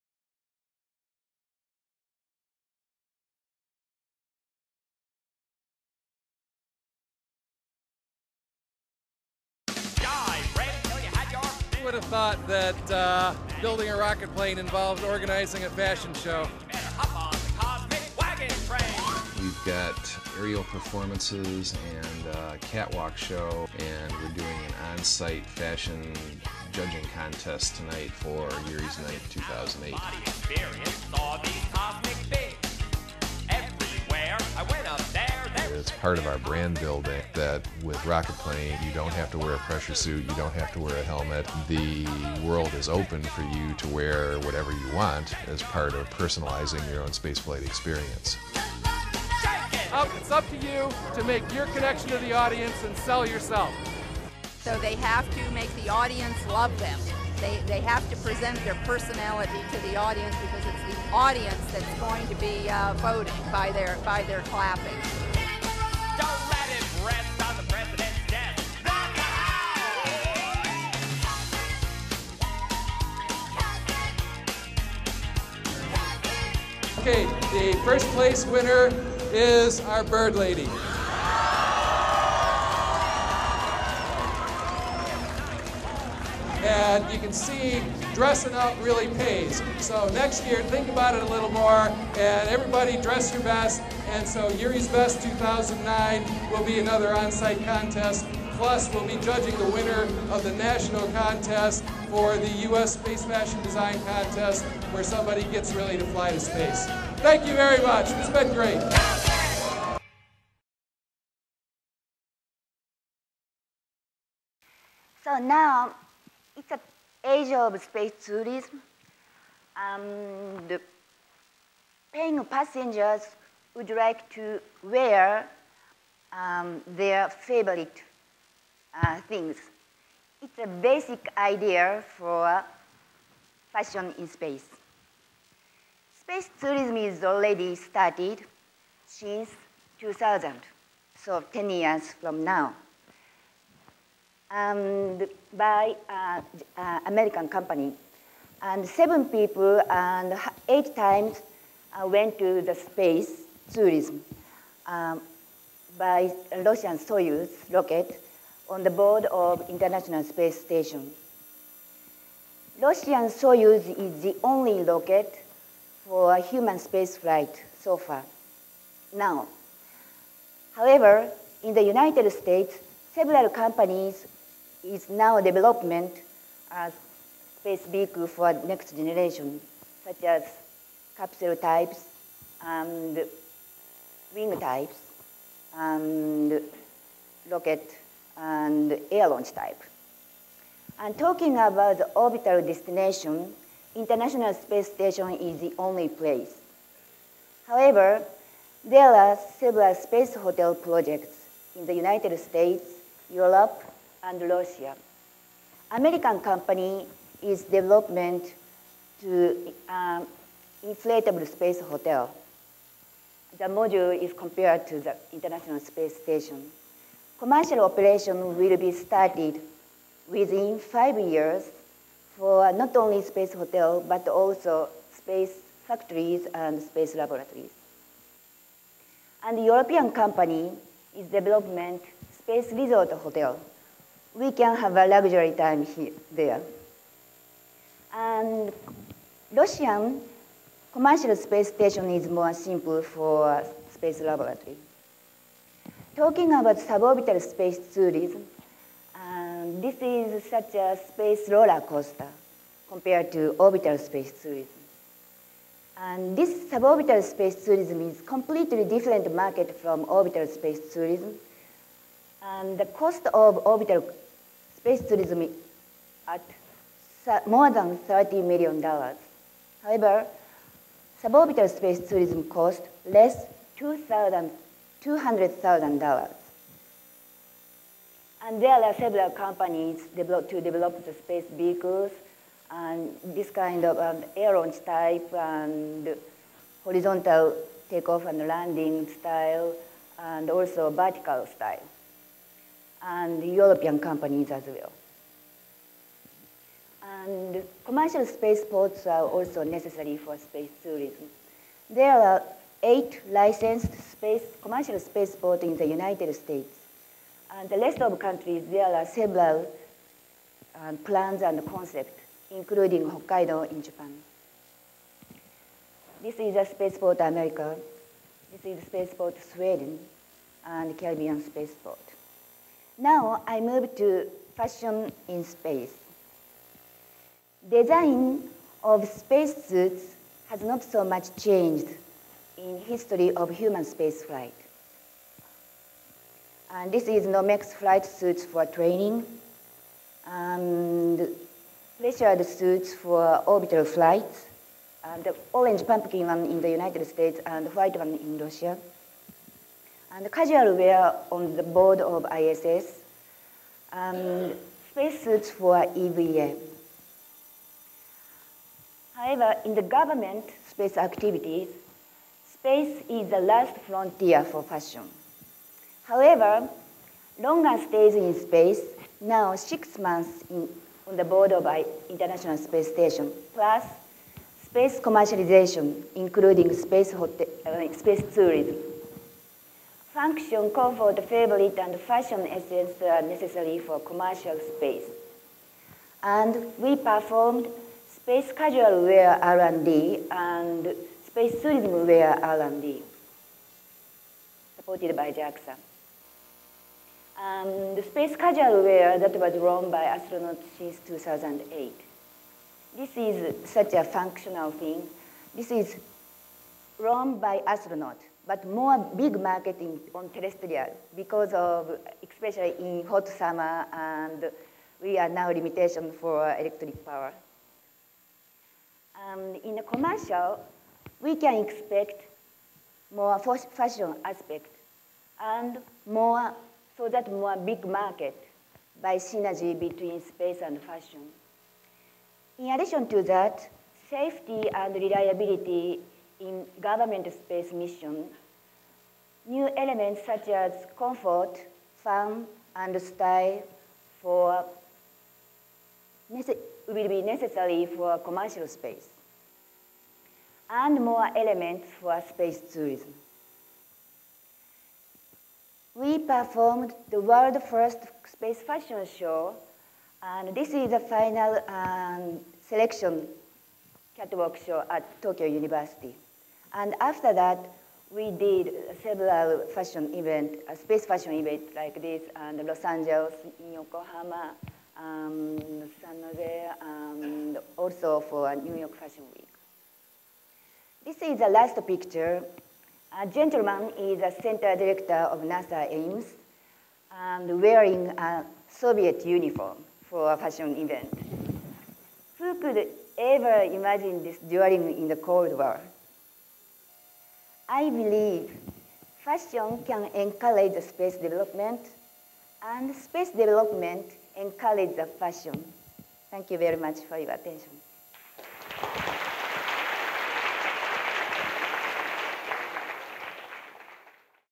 Fashion in Space: Misuzu Onuki at TEDxOsaka
Misuzu Onuki has been an innovator in the development of space tourism and space commercialization research, and she has created space cultural projects such as space fashion and space weddings.